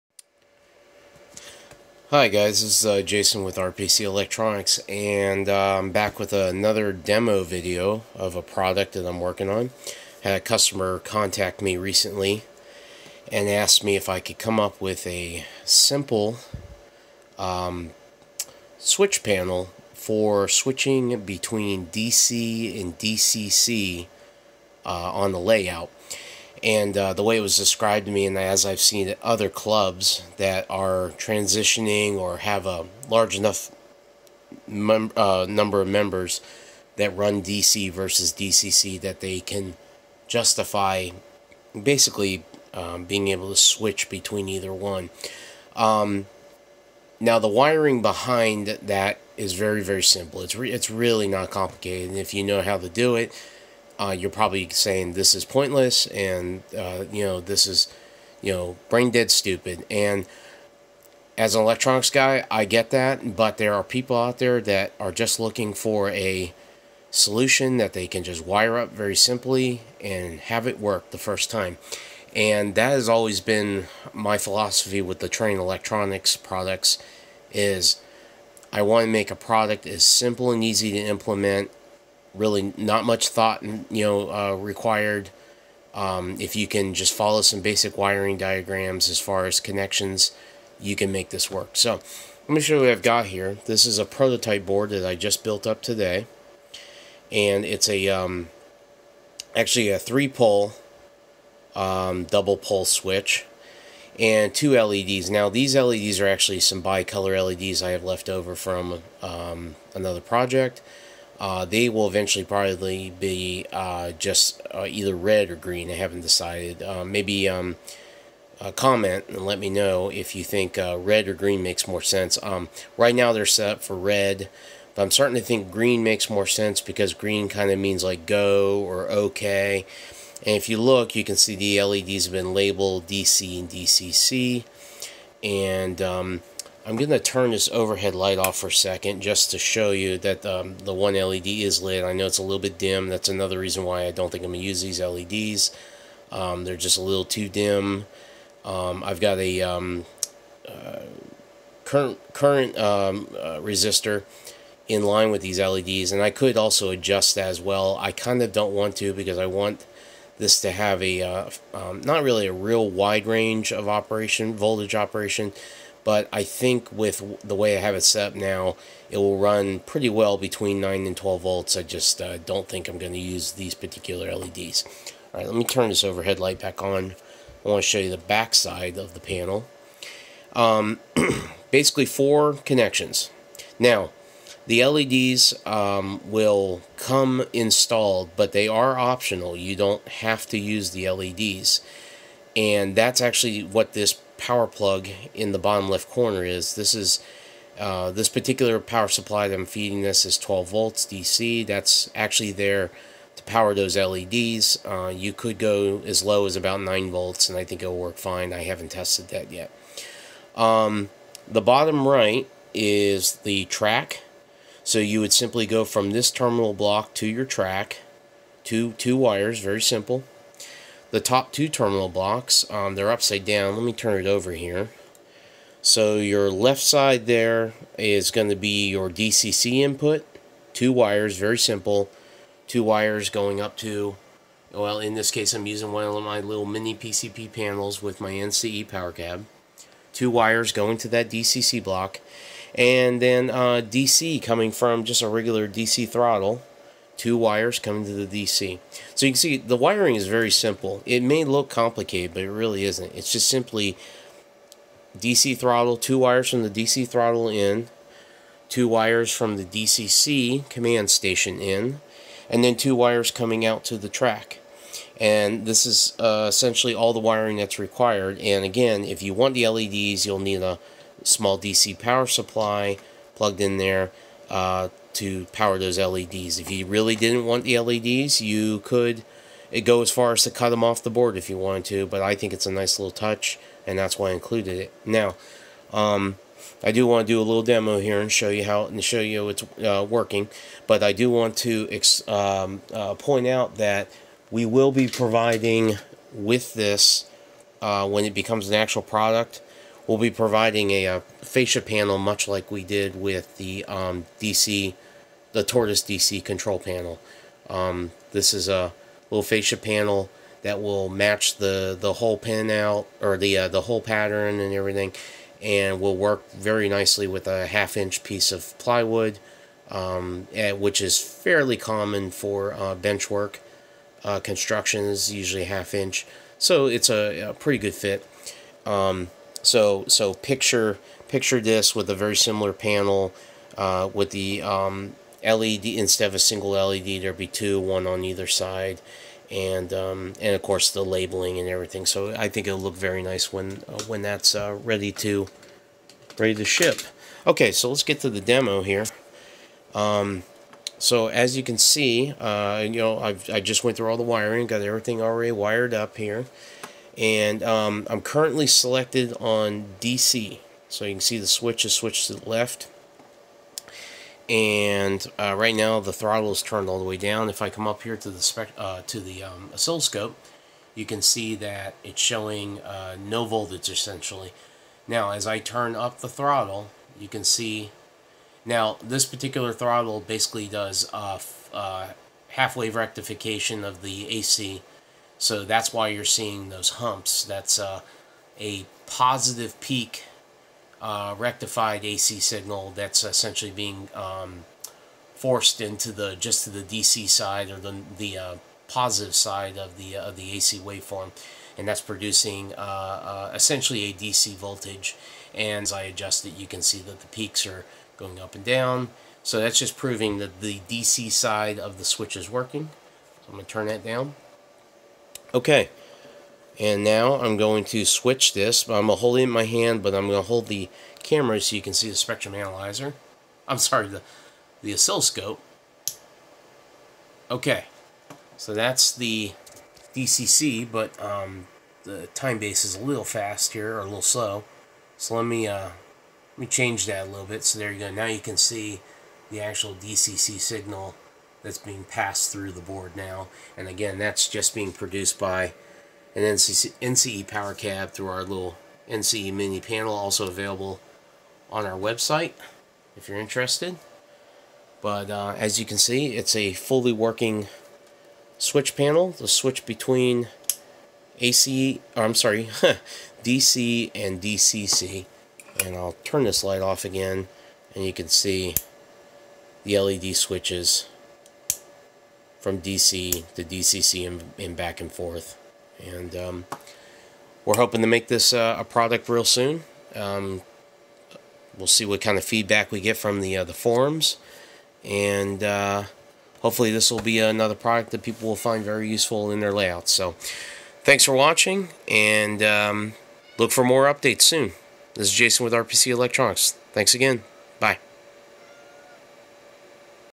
DC/DCC Switch Panel Demo
In this video I'm demonstrating the prototype of our new DC/DCC switch panel.